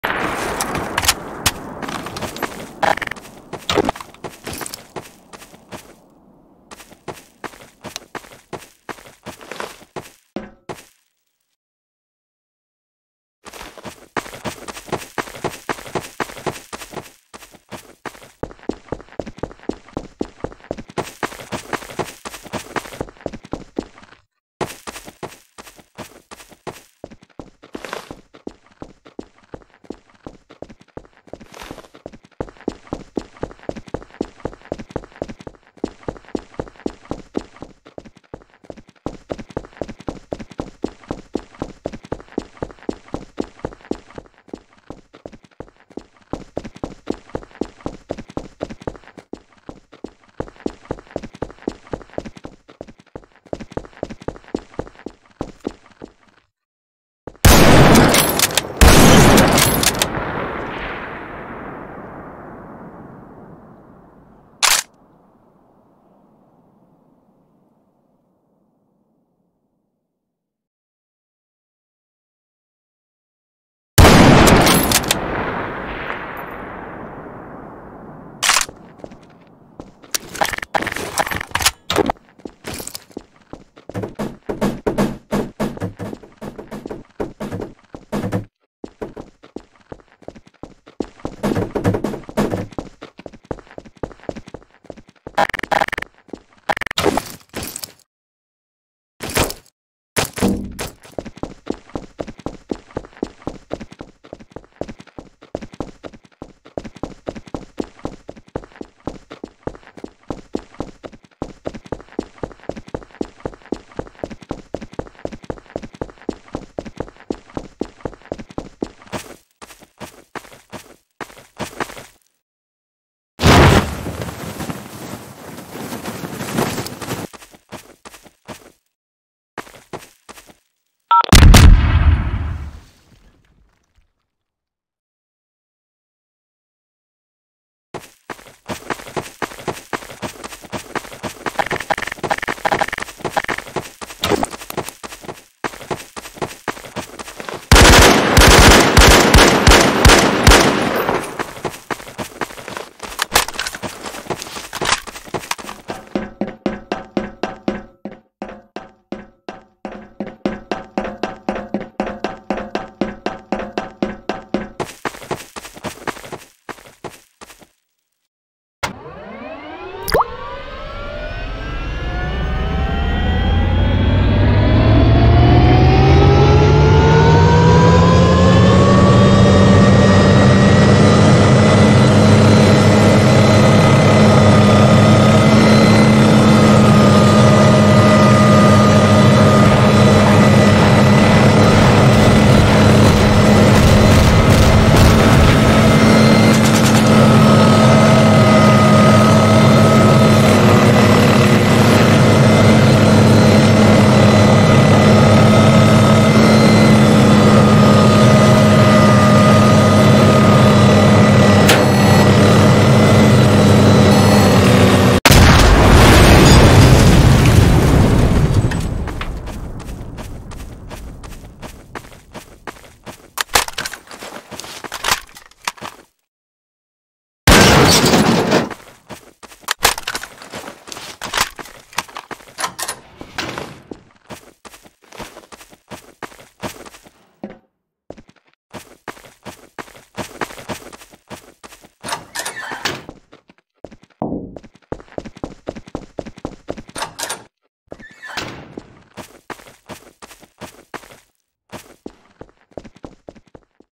russian VIP killed in the area surrounding Zaporizhzhia Nuclear Power Station
Badass Links.

foster valkorov Team leader of the SSO himself goes on a mission to infiltrate the area near Zaporizhzhia Nuclear Power Station and kill a Russian VIP in the occupied area in Ukraine then (in the next video) he and his squad will move in on Zaporizhzhiaa NPS and try to prevent the Russian federation from causing a nuclear meltdown within the reactor core. (THIS IS FICTIONAL AND IS BASED ON THE REAL THING BUT IS DRAMATICALLY CHANGED FOR YOUTUBE PURPOSES SO DONT TAKE THIS SERIOUSLY AT ALL PLEASE) SLAVA UKRAINI!